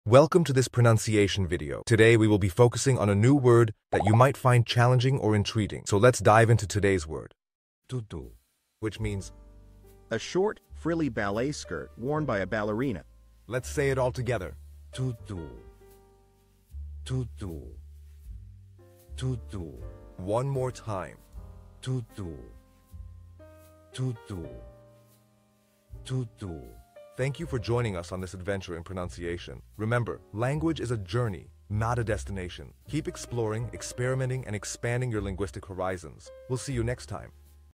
How to pronounce Tutu
Master the Pronunciation of 'Tutu WHICH MEANS TUTU' - which means : The short, Frilly Ballet Skirt Worn by Ballerina. 📝 with @PortuguesePronunciation 🎓🗣️ - [Your Guide to Portuguese]

Learn to pronounce 'Tutu' perfectly in Portuguese with our easy and engaging tutorial by @PortuguesePronunciation! 📖📽️ In this comprehensive video, we provide you with native speaker examples 🗣️👂 and phonetic breakdowns 🎵 to ensure you get the pronunciation just right.

How to say Tutu in Portuguese